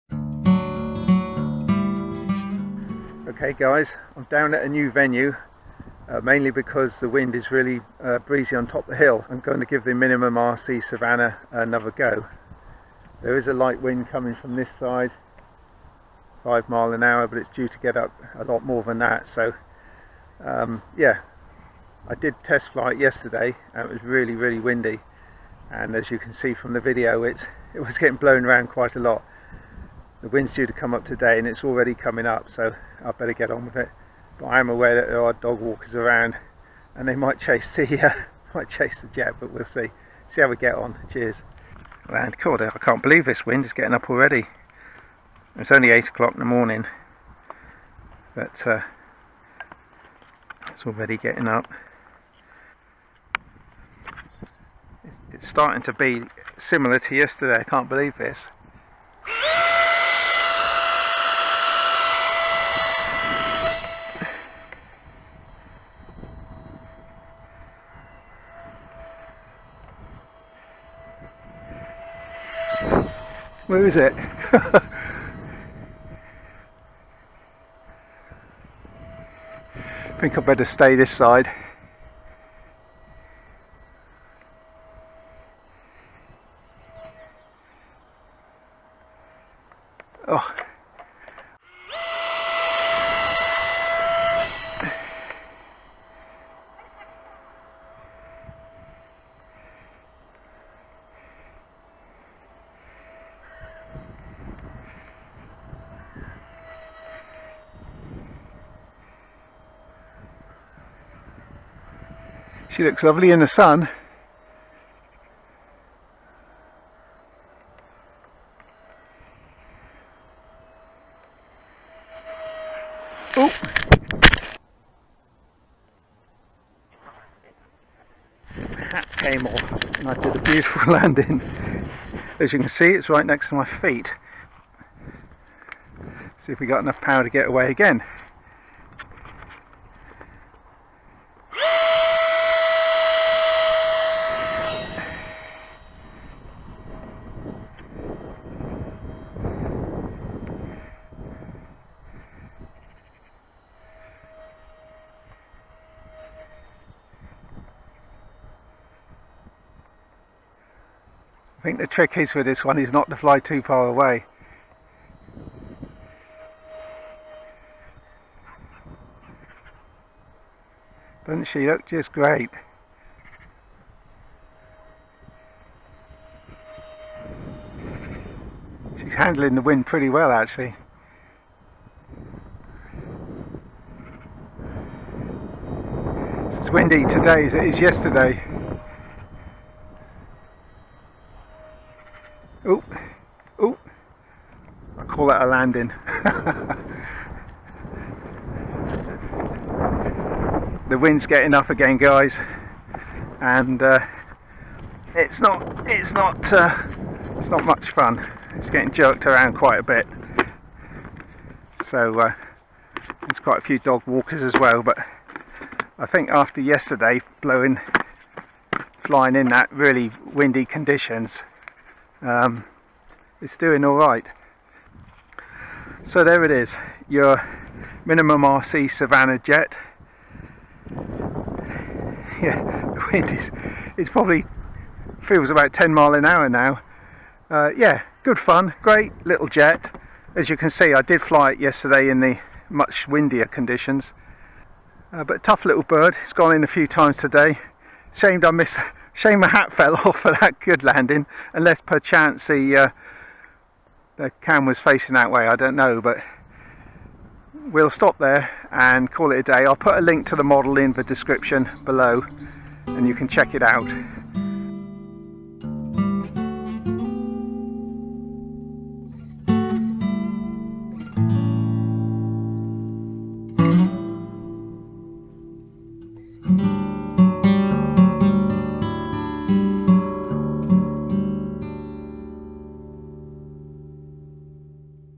Savannah EDF Jet Maiden Flight
The Savannah ducted fan jet from MinimumRC gets two bites of the cherry for a maiden both days not being really suitable but not to bad considering the conditions. check out the model
SUPPORT 'Cliff Harvey RC Planes' and JOIN for EARLY ACCESS to NEW videos